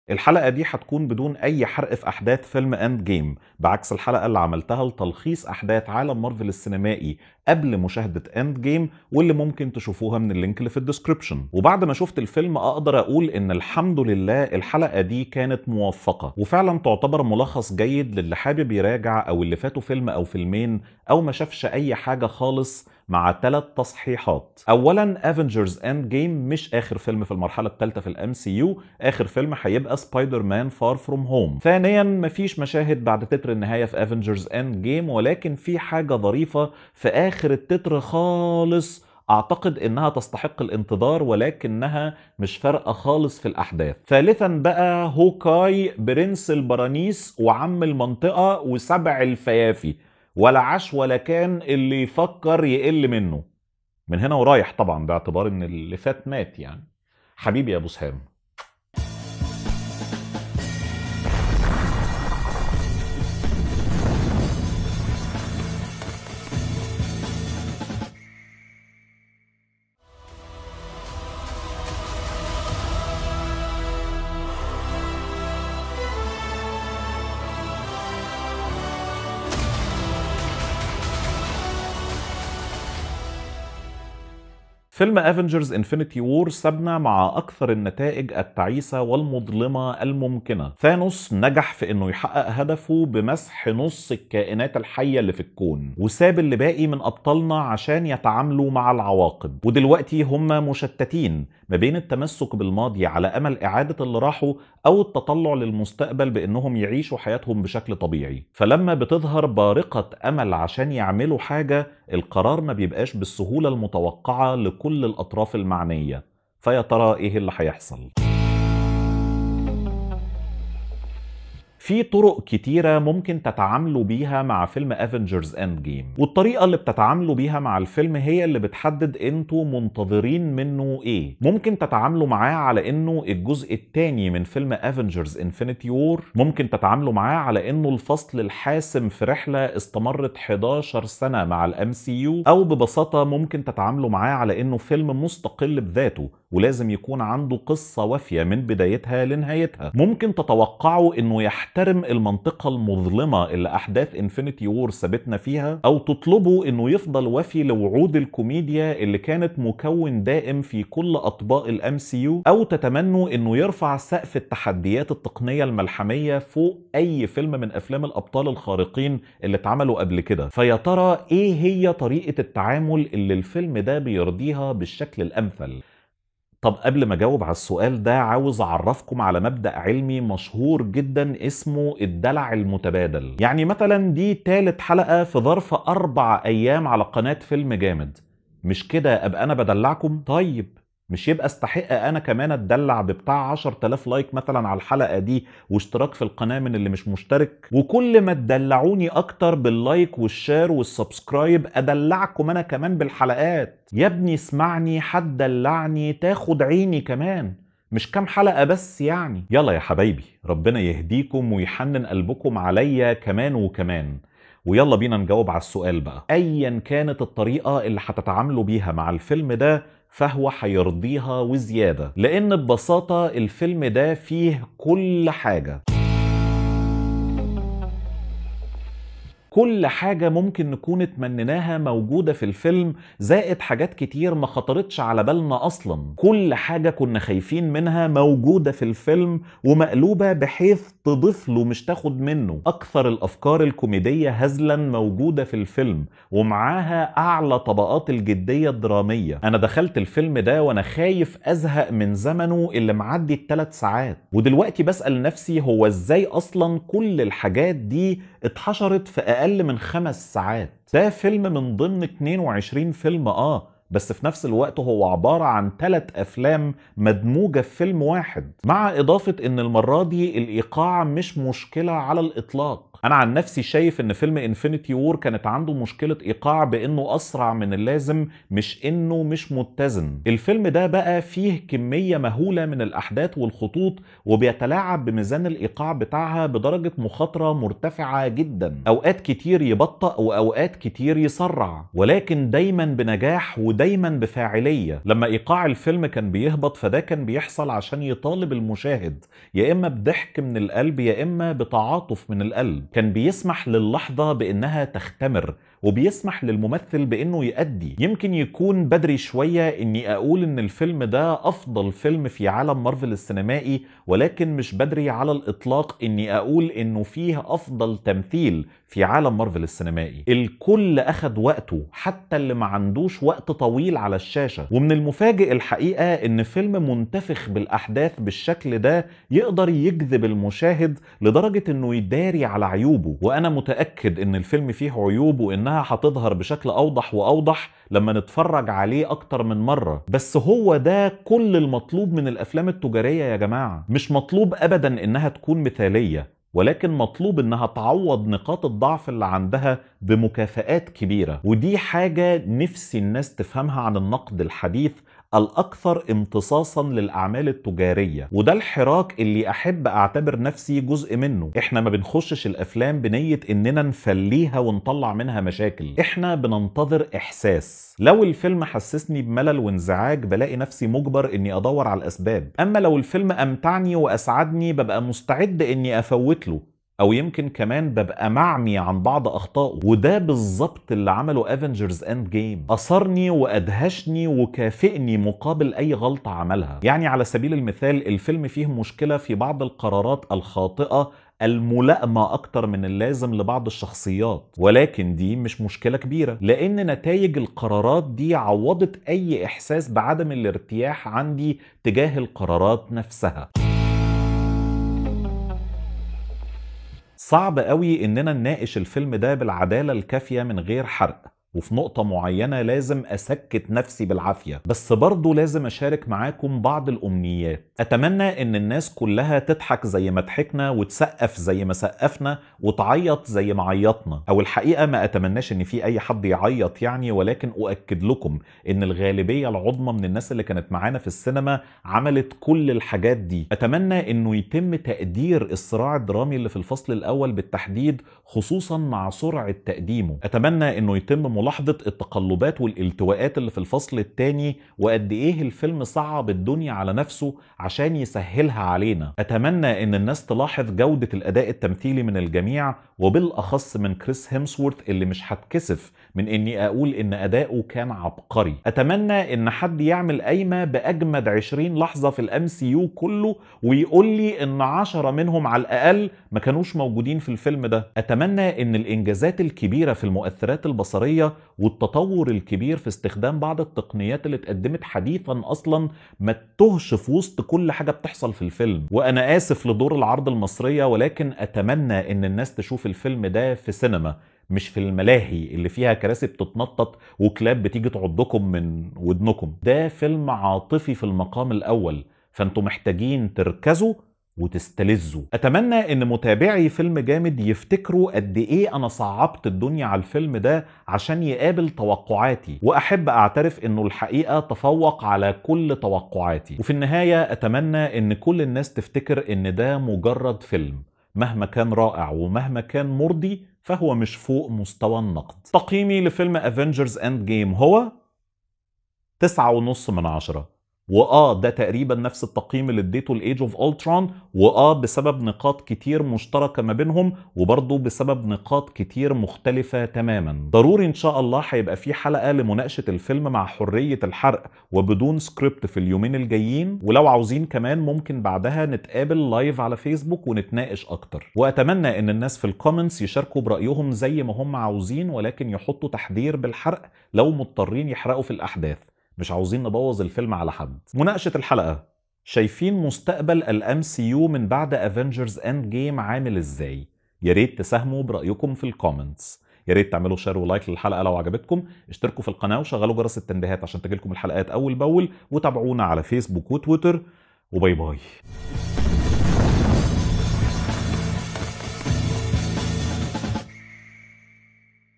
مراجعة بدون حرق لفيلم Avengers: Endgame | فيلم جامد | FilmGamed
مراجعة بدون حرق أحداث لفيلم Avengers: Endgame من فيلم_جامد .. الفيلم رقم ٢٢ بين أفلام عالم مارفل السينمائي والجزء الثاني المرتقب من فيلم Infinity War. الفيلم من بطولة روبرت داوني جونيور وكريس إيفانز وسكارليت يوهانسون ومارك رافالو. السيناريو لكريستوفر ماركوس وستيفن ماكفيلي والإخراج لأنتوني وجو روسو.